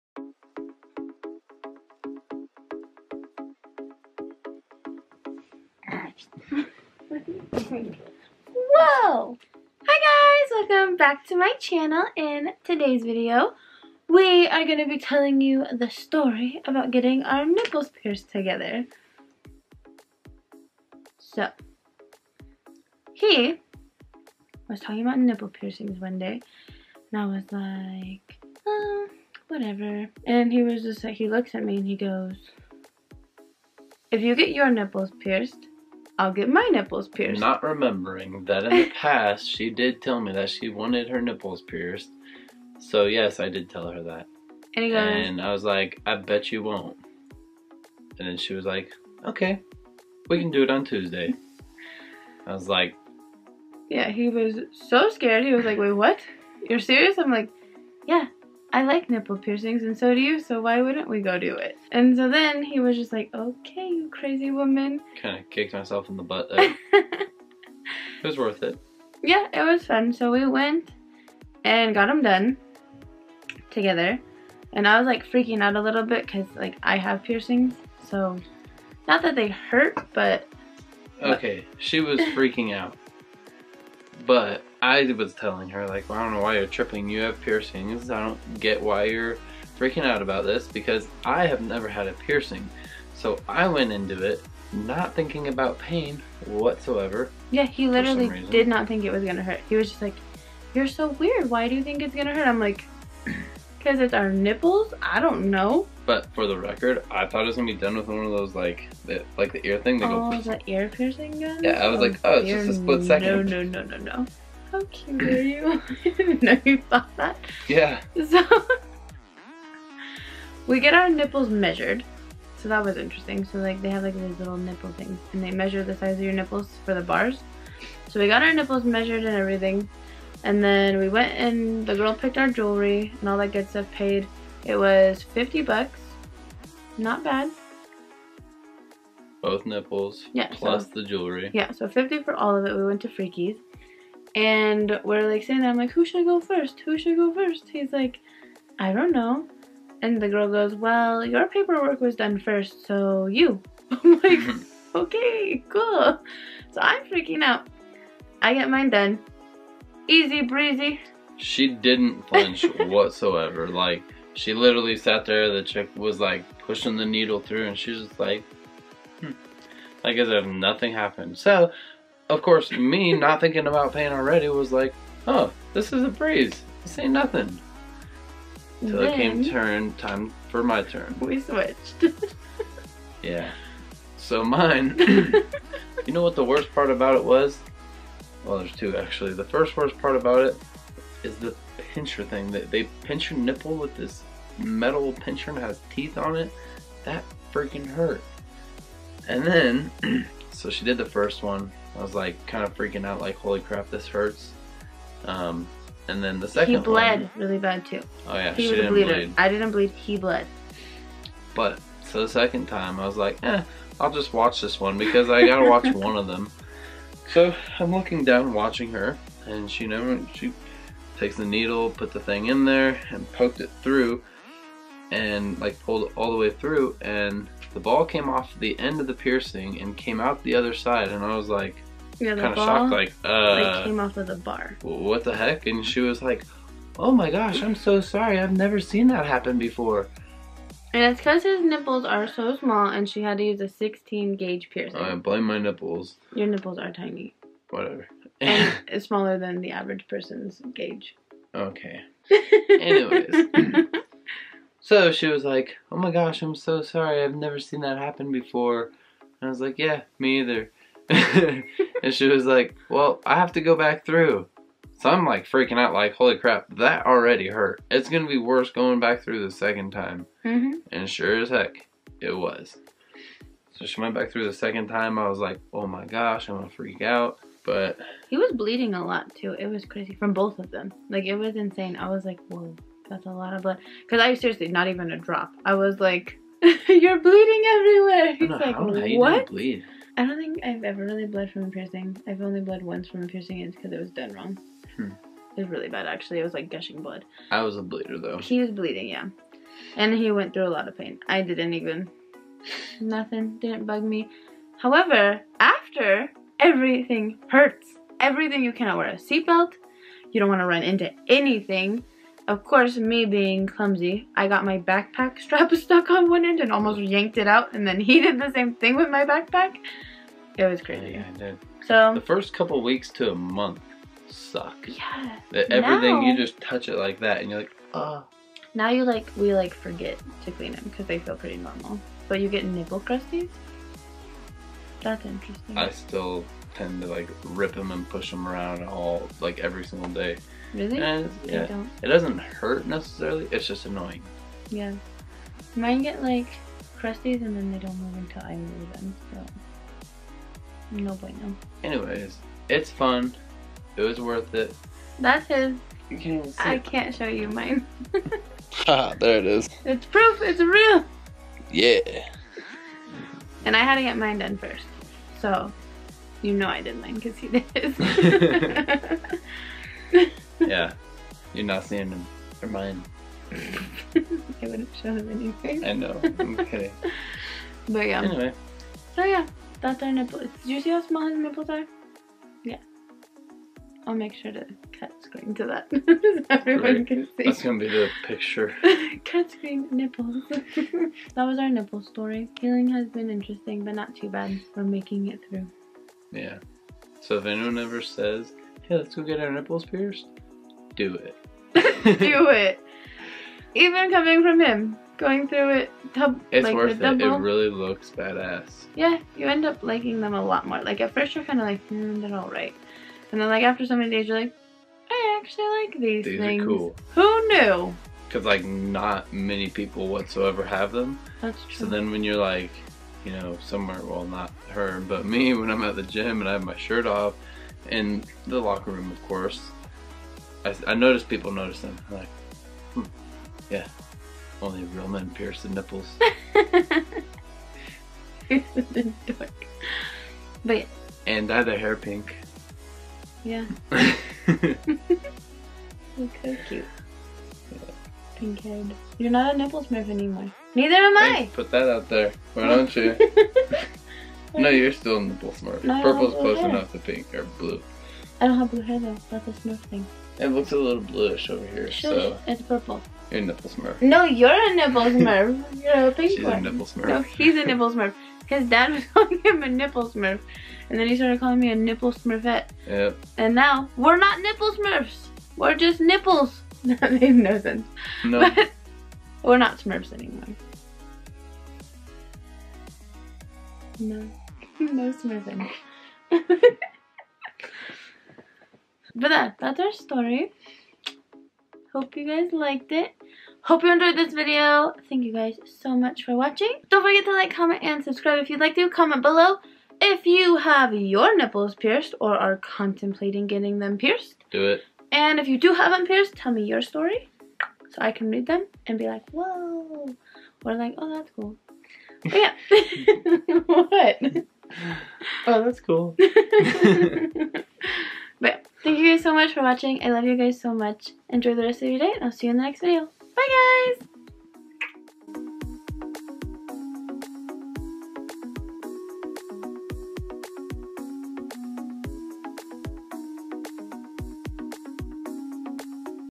WE GOT OUR NIPPLES PIERCED!!! | Makayla Wetmore
Hey Babes, I hope you guys enjoy this story time about us getting our nipples pierced. If you want to know our experience check out this video!!!

Clothing - Forever 21
Plugs - Love Kills Boutique
Hair Color - Pravana

Facebook.com/makaylawetmoremodel